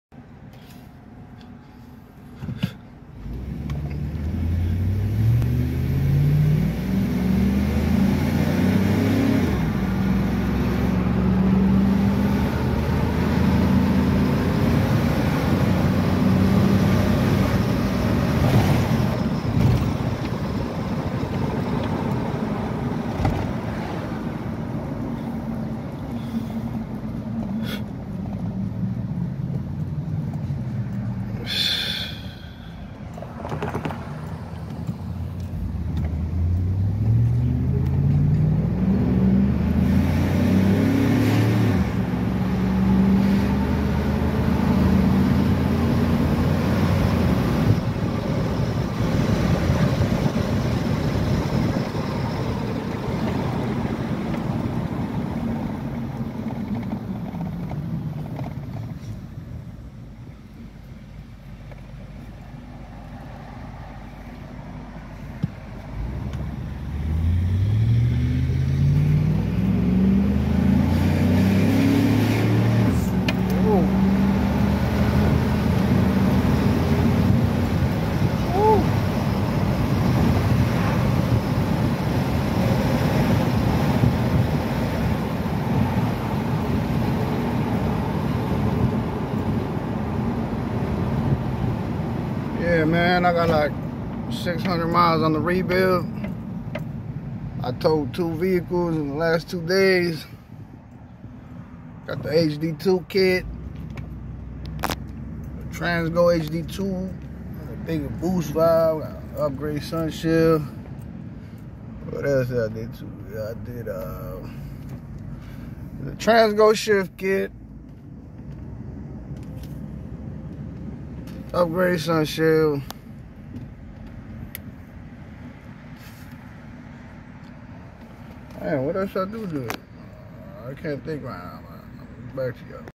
4l60e Rebuild Run. Tahoe 2wd 4.8 ls Transgo HD2, hardened sunshell, 9 clutch 3-4 stack,
fully rebuilt 4l60e on my 03 tahoe 4.8. replaced all bearing bushings seals sealing rings  transgo hd2 shift kit upgraded sunshell 500 boost valve , new reaction shaft new pinions sprags. transgo separator plate drilled out.  firmness level 1 . new torque converter. dexron 3 shifts realllllyyyyy nice would definitely recommend the kit.